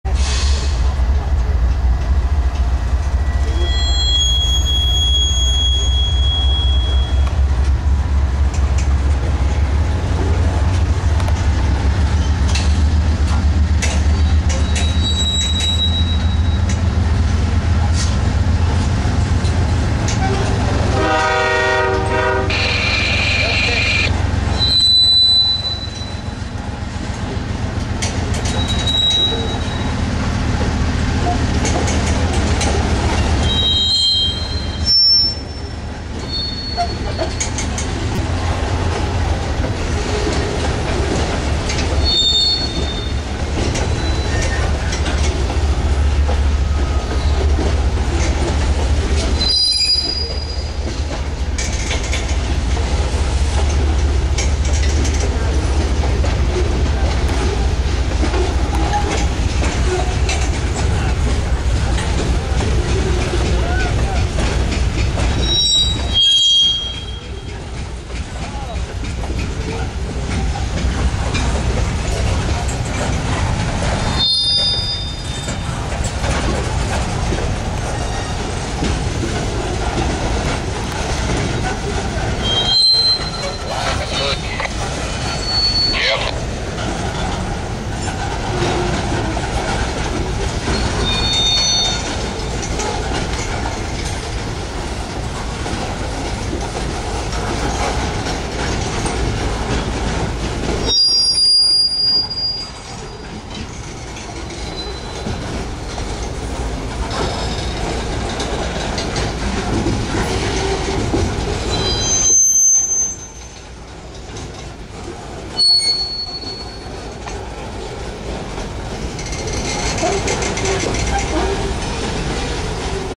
My second trip to Irondale 2/7
I didnt have a video of this trains but I started to video tape it because of the slack action & a NW coil cover